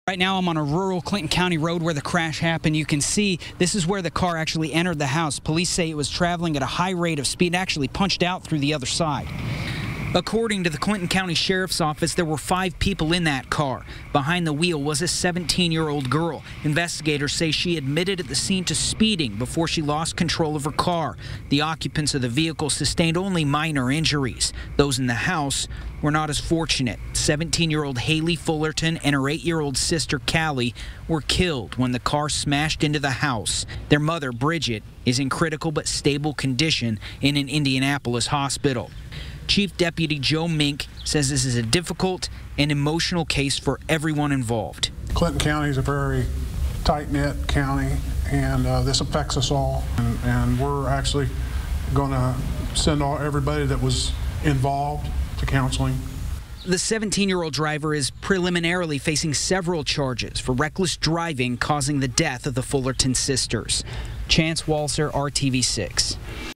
Teen driving vehicle that crashed into Clinton Co. home killing two kids arrested
A 17-year-old girl who was driving a vehicle when she crashed into a Clinton County house, killing 2 kids Wednesday evening has been arrested.